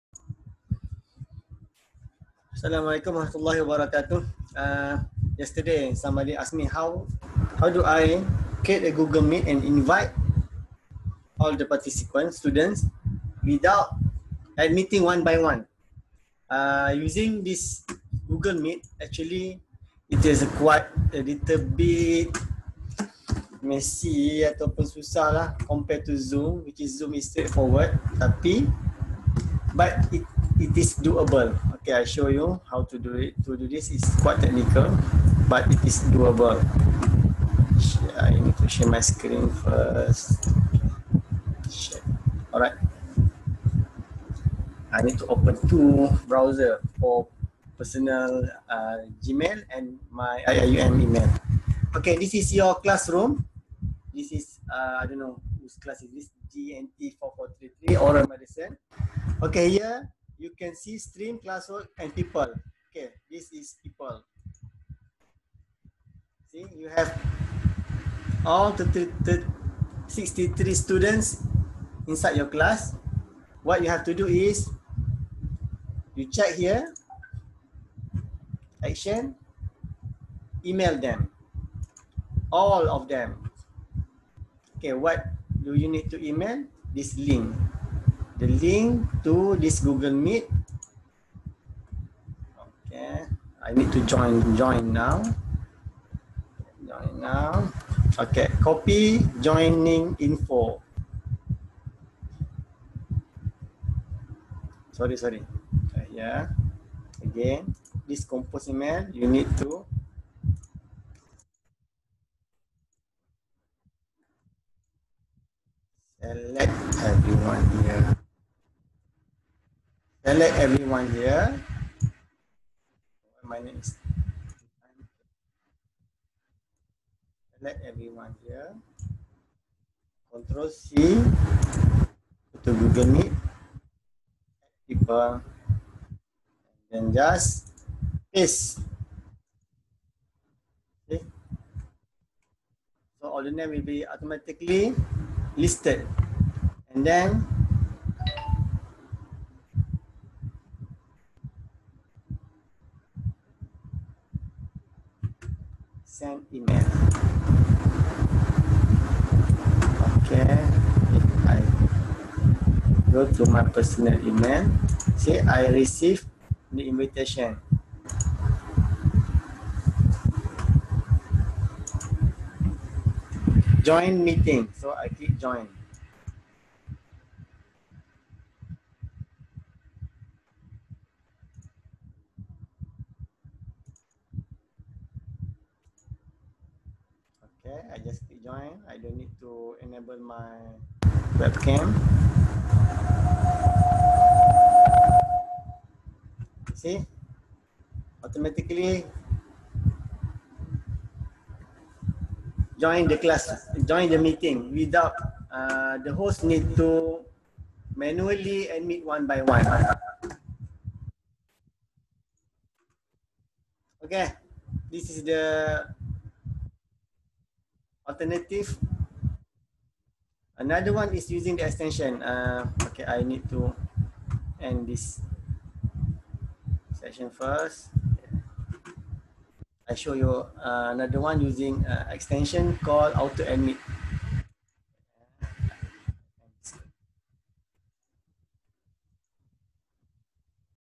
Google Meet. How to add participants without manually admit.
Unlike Zoom, GMeet need to invite all the participants using their personal email and then the host just waiting for them to join. No need to click admit 1 by 1.